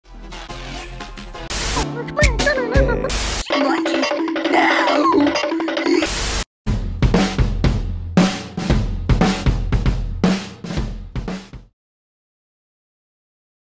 The New! (And improved) The Stoopids intro!
Your gonna be seeing this before a stoopids episode from now on!